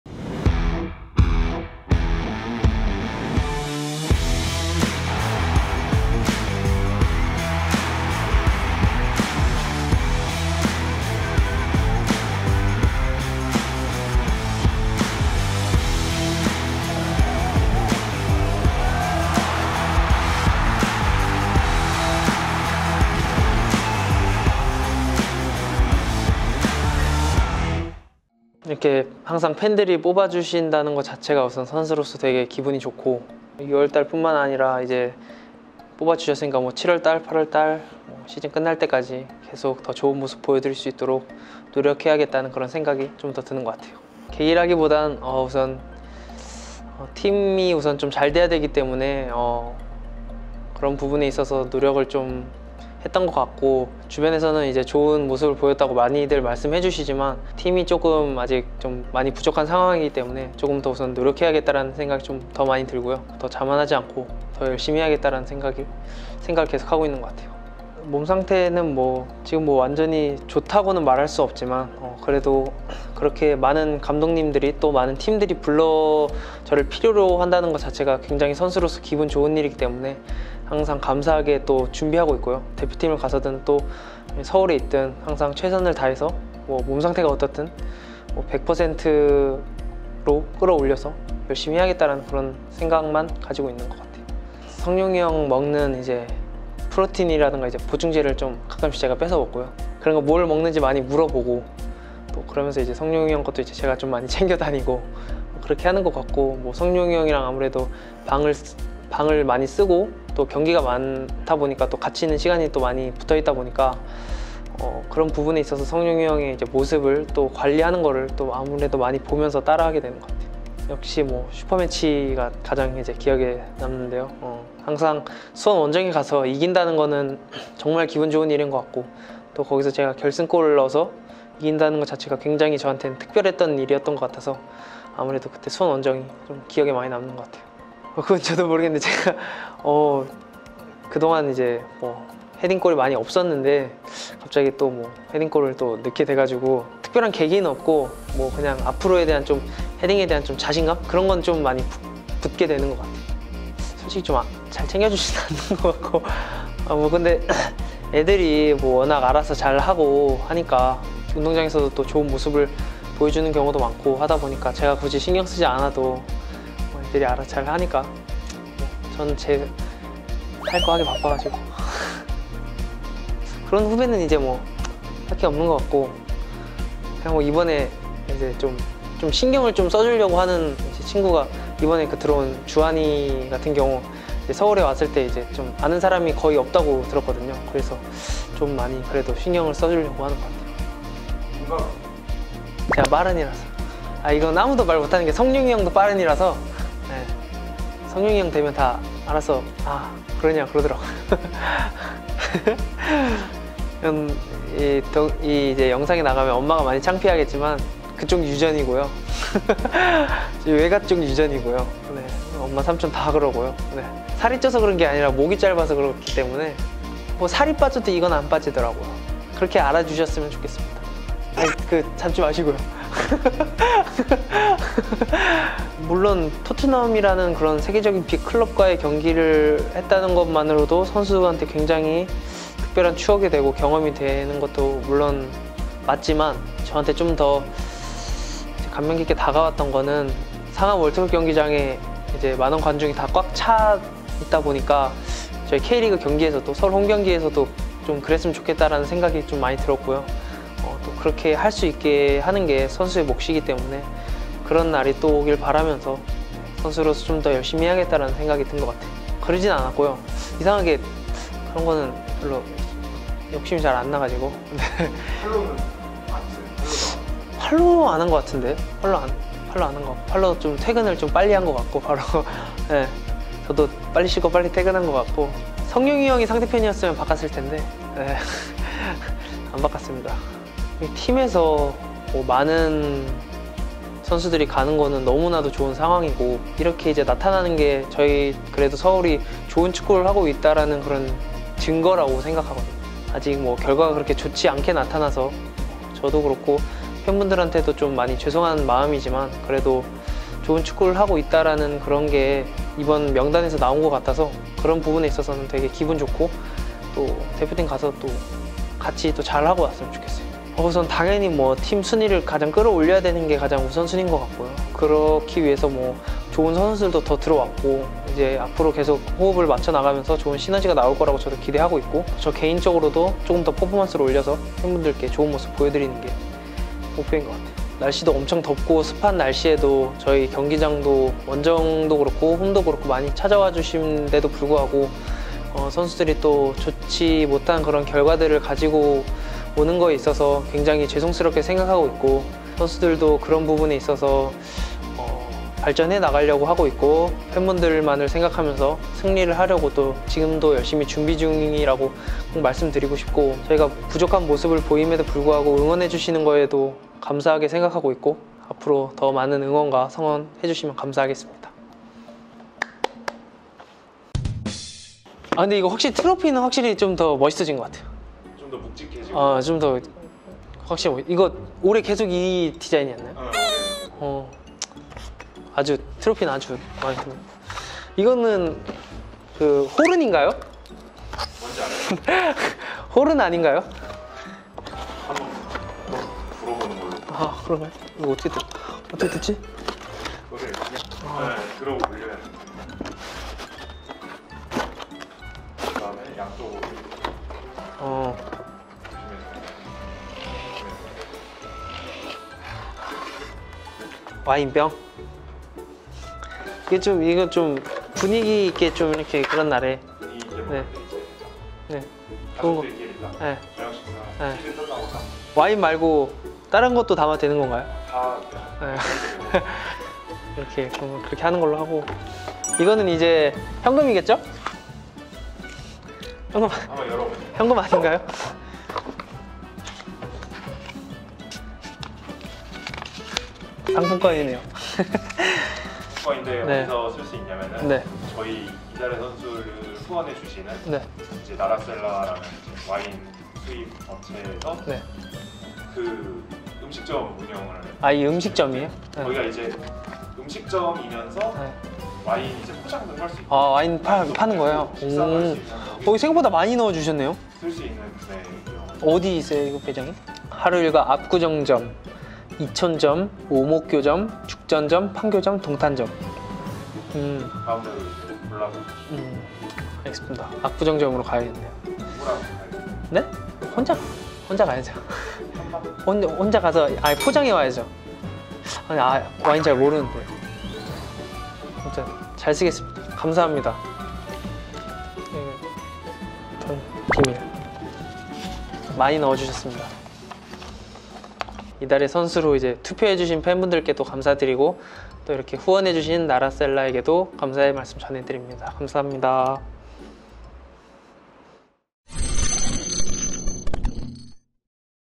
나라셀라 6월의 선수 | NO. 11 조영욱
와인 문화를 선도하는 기업 나라셀라와 함께하는
‘나라셀라 6월의 선수’ 수상자는 조영욱 선수입니다.

100% 팬 여러분들의 투표로 선정하는 나라셀라 이달의 선수에
앞으로도 많은 관심 부탁드립니다!